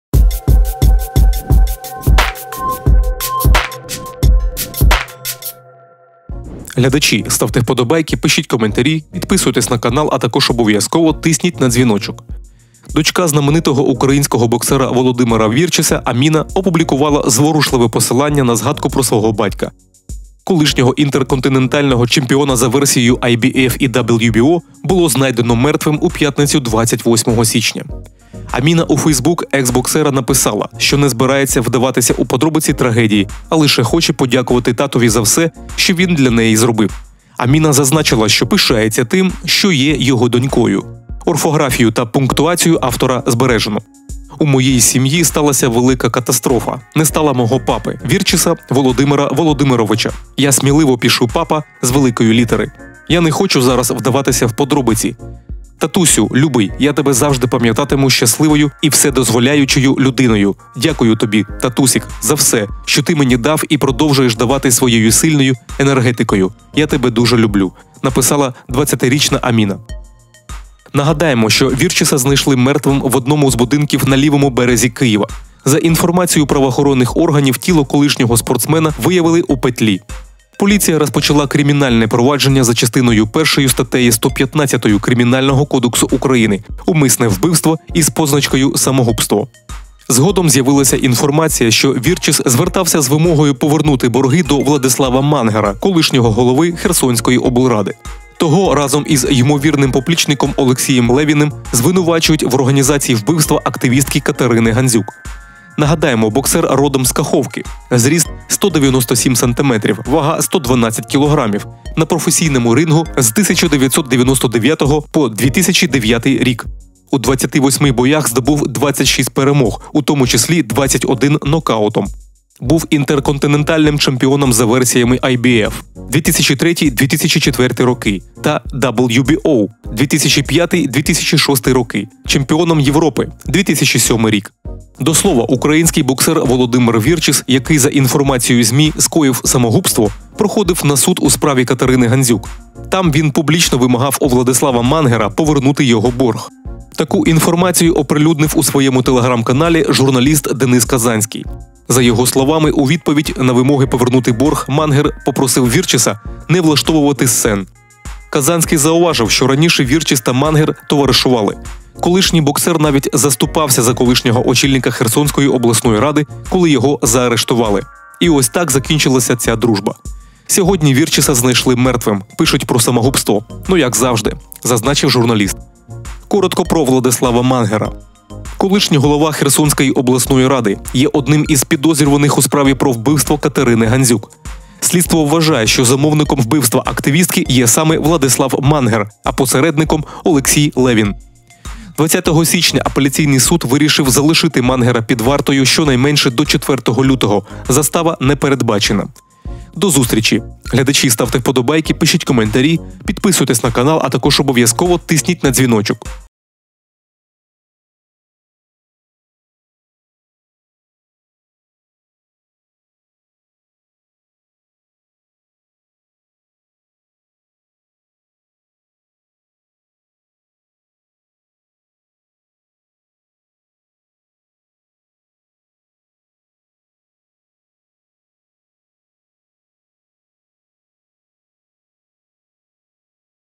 Донька в траурі! Папи не стало: страшна звістка. Мангер попав, відразу після суду вирішилось
Аміна у Facebook ексбоксера написала, що не збирається вдаватися у подробицi трагедії